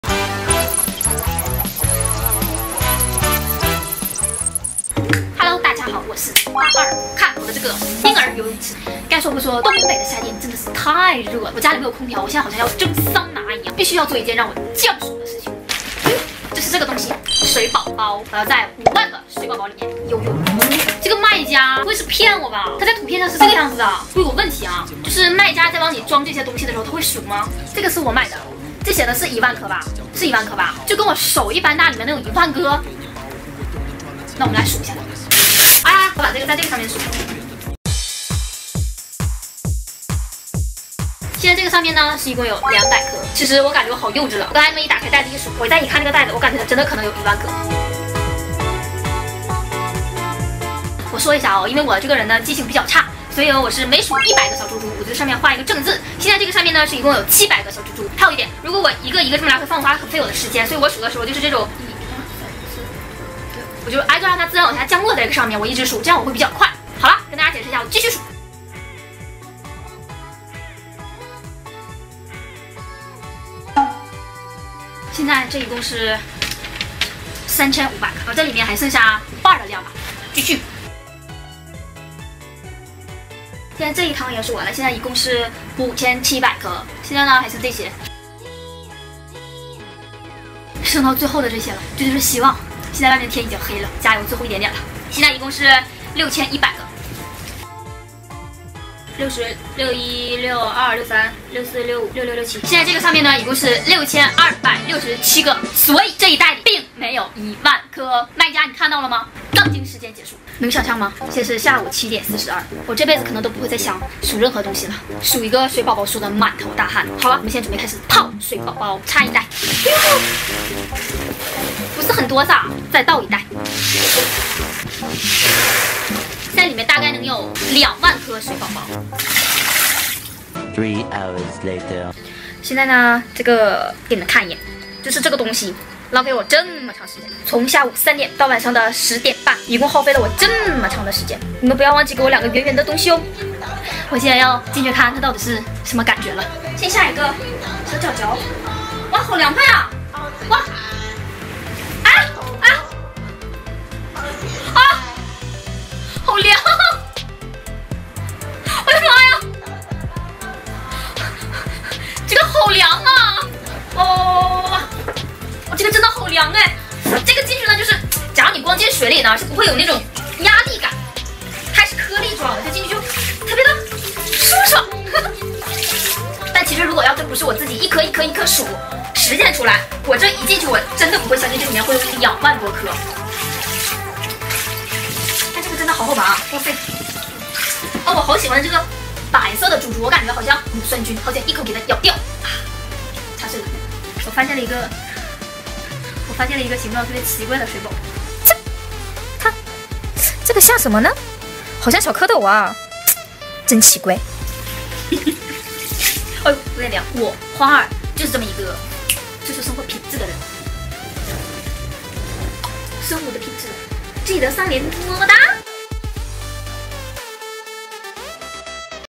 怒数10000个水宝宝！杠精质疑店家数量问题！结局最后？？？
所以 这个水宝宝属于什么垃圾？
在线等 有点急！
数的眼睛都花了 记得三连呀！么么哒！